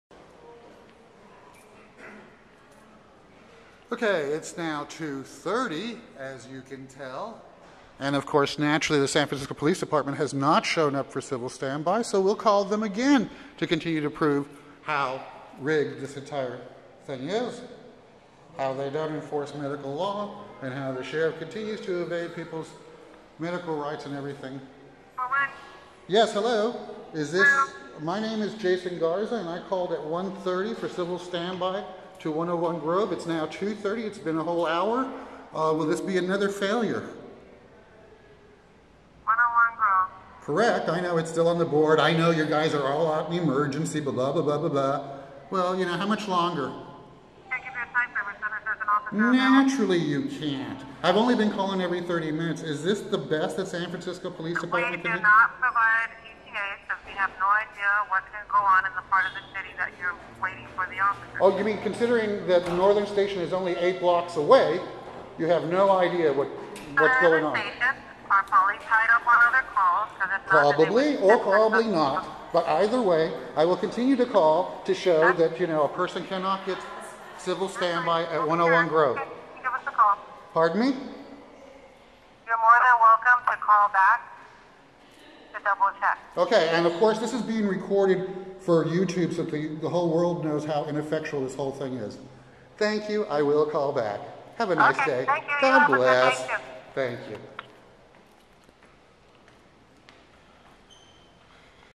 012016p5 DPH Sheriff DEM SFPD BULLSH*T - NO MEDICAL LAW ENFORCEMENT
Here is a VIDEO where SF is PLAYING the COMMUNITY and I ask the relevant QUESTION ... DO YOU KNOW THAT SFPD DOES NOT ENFORCE MEDICAL LAW ... watch his response when I point out that if they DO NOT ENFORCE MEDICAL LAW then that leaves patients on the streets to be shot by SFPD ... sort of like putting the CART before the HORSE ... oh, and for your information ... the CAPTAIN DOES NOT ANSWER ... SIMPLY WALKS AWAY.